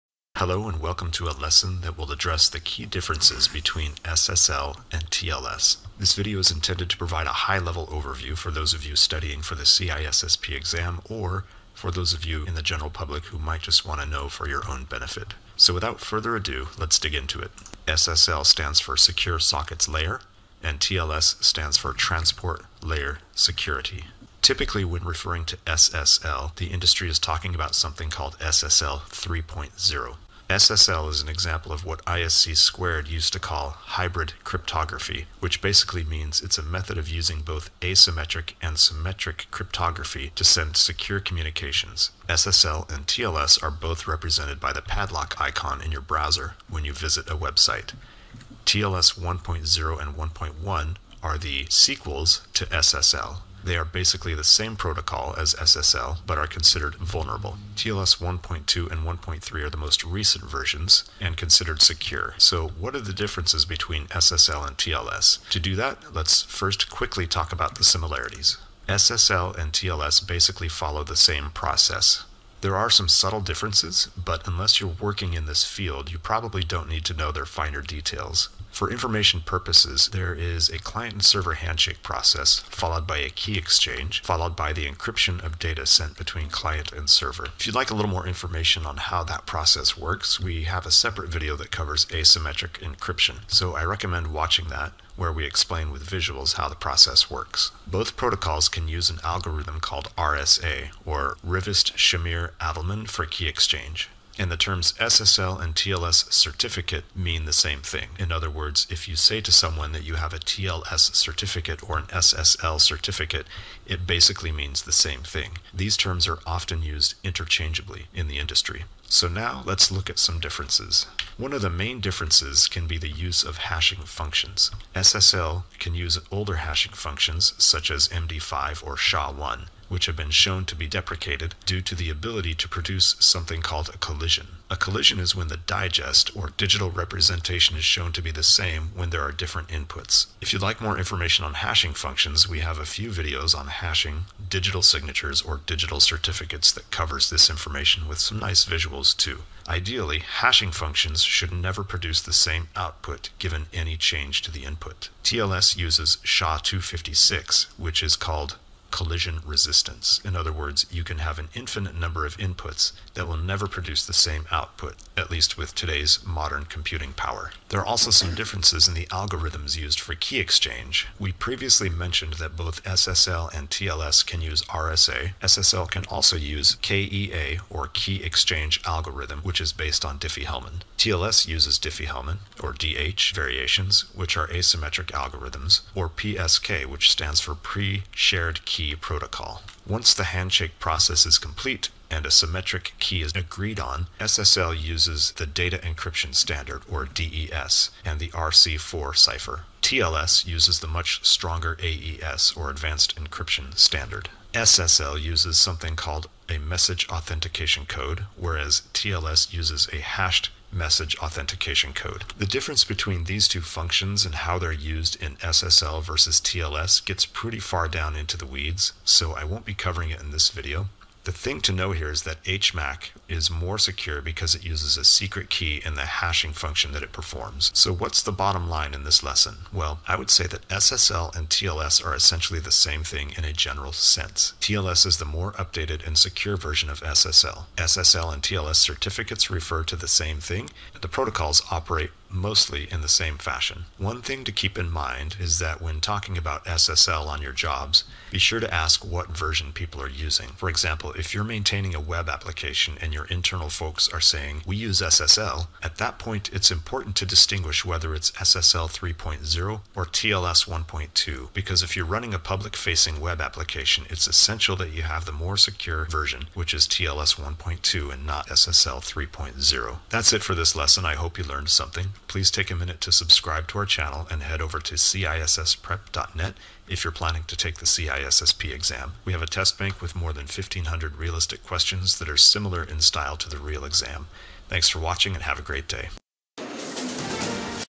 SSL vs TLS (Fixed Audio) What's the Difference?
This video covers the basic differences between SSL and TLS.  Since the focus of this channel is on preparing for the CISSP exam, we won't go into too much detail.  This is just to give you an overview.

This video is for those of you who complained about the alleged "bass boost" in my first video on SSL vs. TLS.   Apologies for the delay, I've been very very busy with the CBK update...